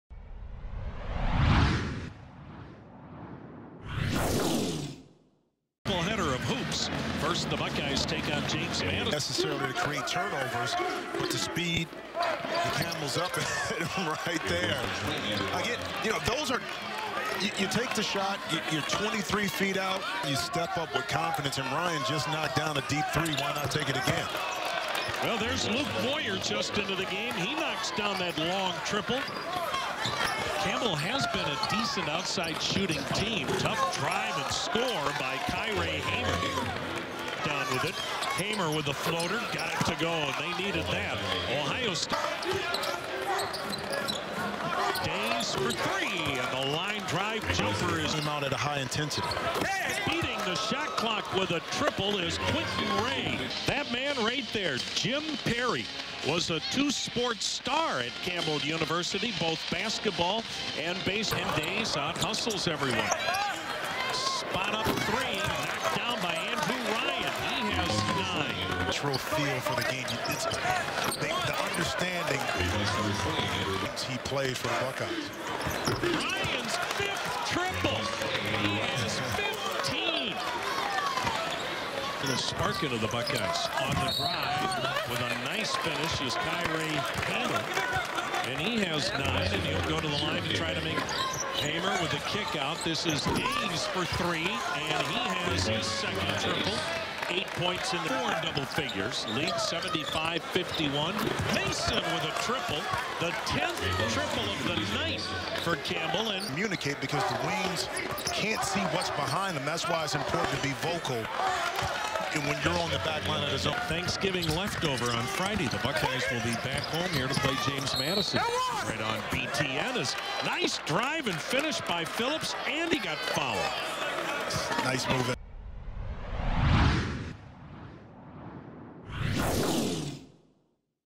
Campbell Men's Basketball at Ohio State - 11/26/14
Highlights of Campbell University Men's Basketball at Ohio State.
From 11/26/14